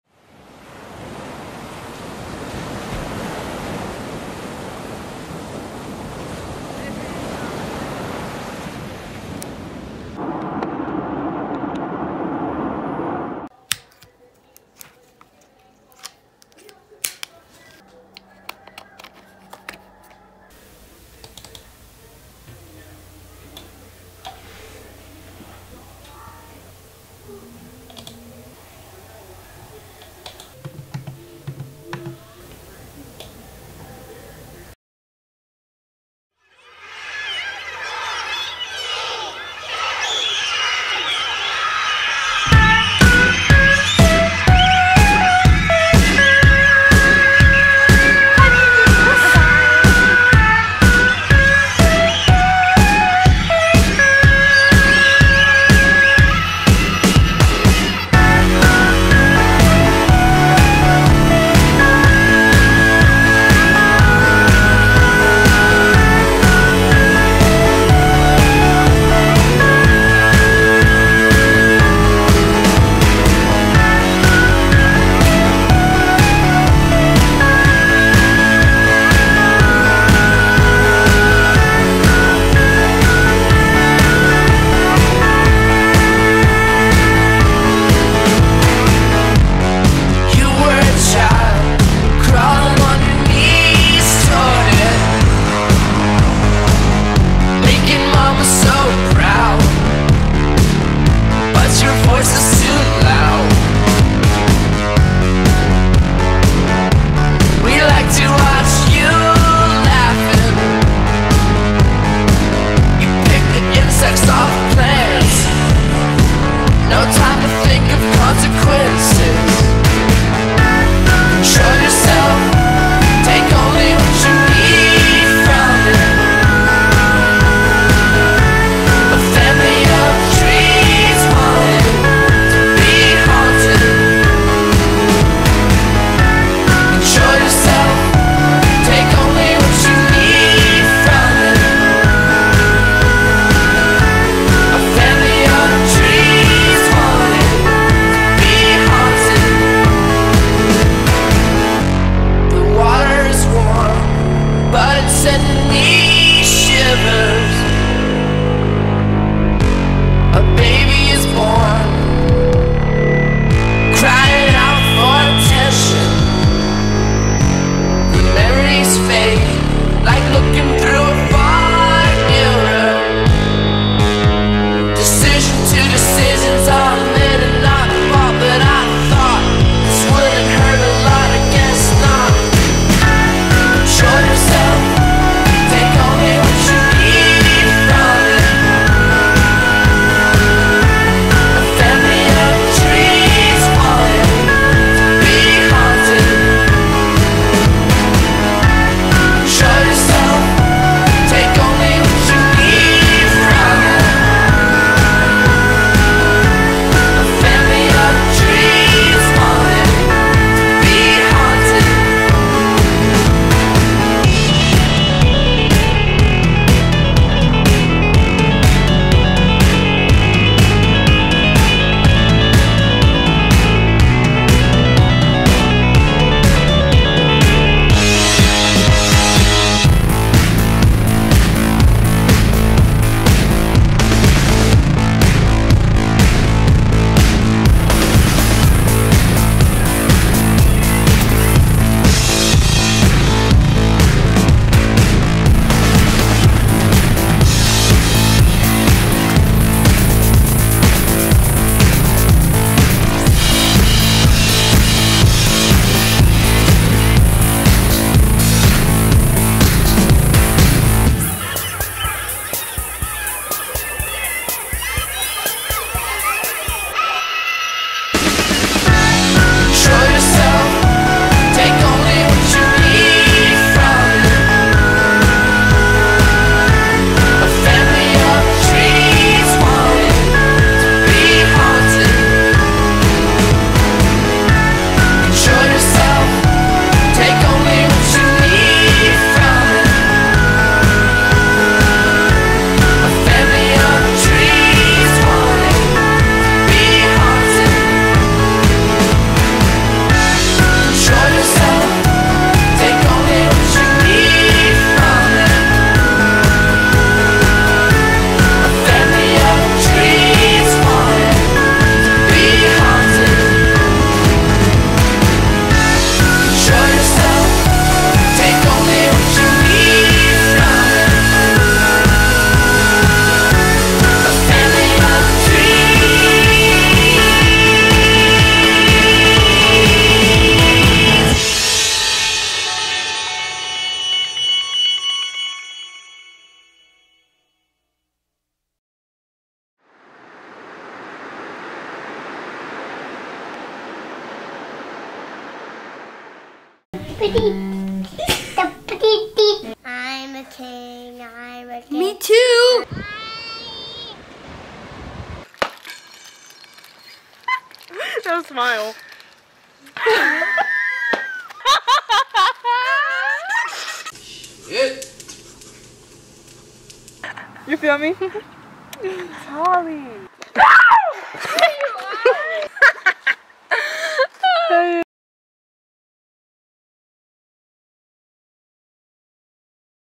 2019: my last year as a kid
It's out! I'm honestly shocked by how fast I got this up haha! This past year has been very memorable, and I for sure will never forget it. Thank you to everyone that made 2019 unforgettable :').